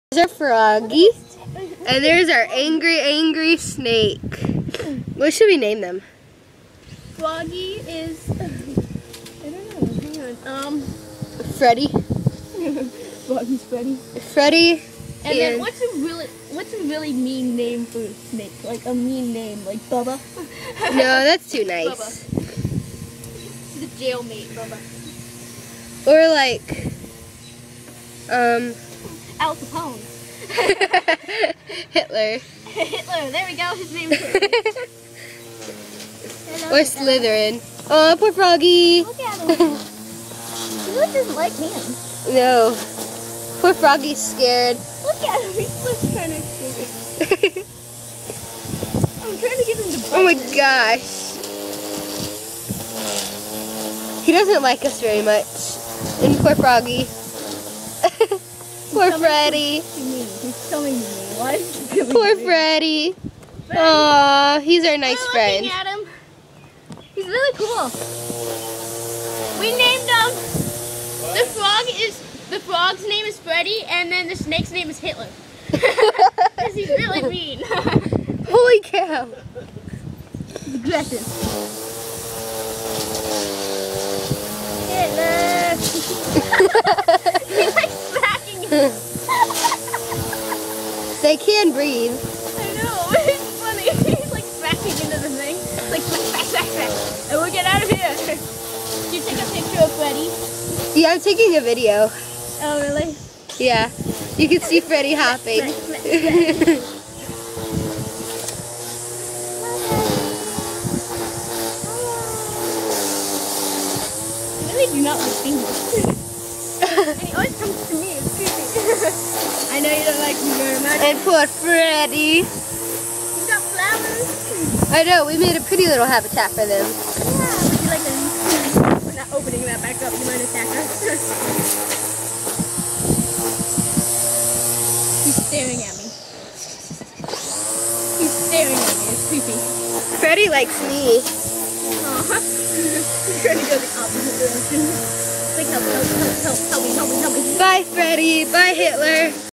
Catching Snake and Toad
Snake= Hitler Hisser
Frog= Freddy Frog
we also found two dead shrews named Jim Bob and Sally Sue the Shrew